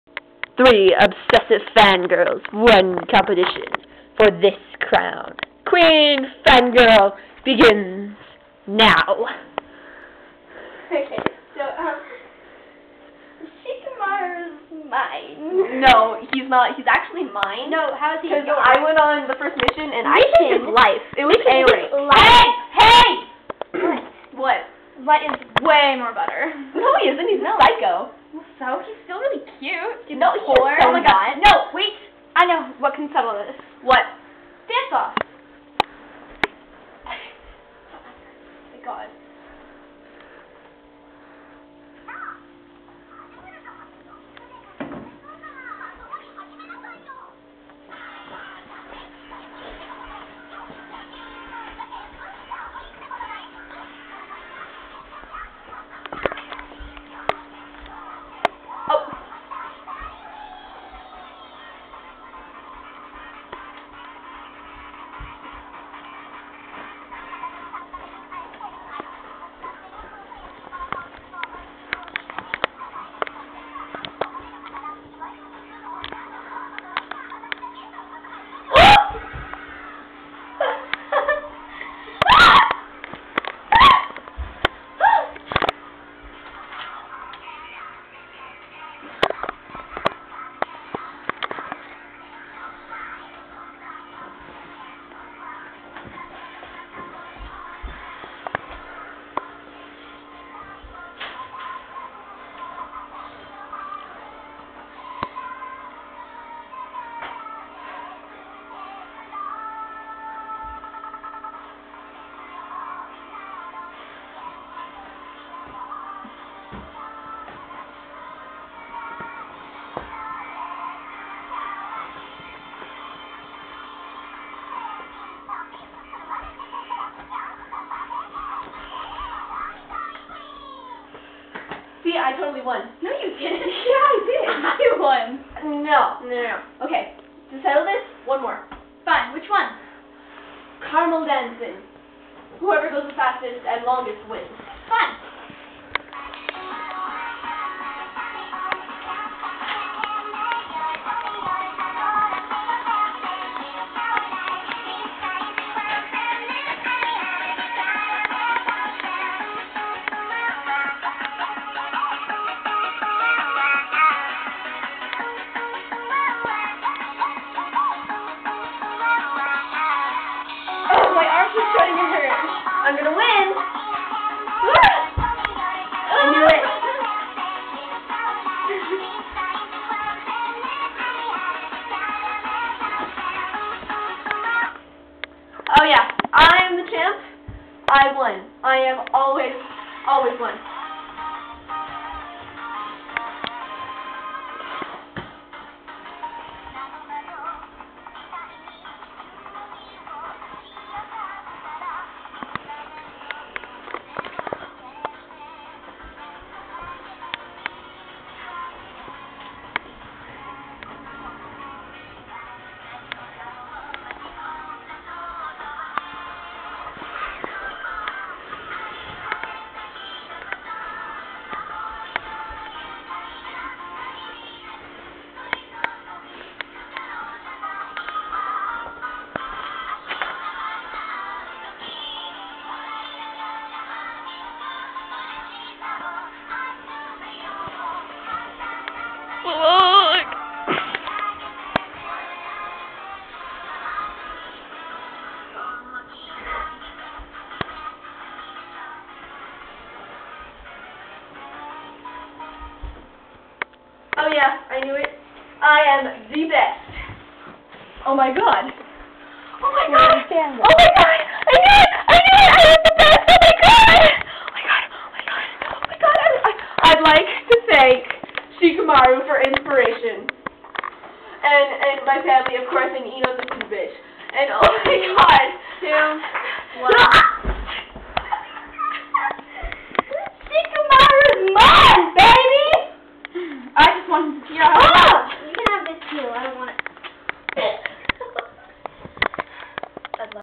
FanGirl Battles
THE FANGIRL BATTLES:
A battle of devotion among some obsessive Fangirls. There will be a lot pain. There will be many tears. And a few broken hearts, but only one winner.
WHO WILL WIN THE FANGILR BATTLES COVETED TIARA?

Answers to Frequently asked questions:
Yes, it was the middle of the night.
Yes, we were hyper.
No, we had NOTHING better to do.
No, we are not on crack.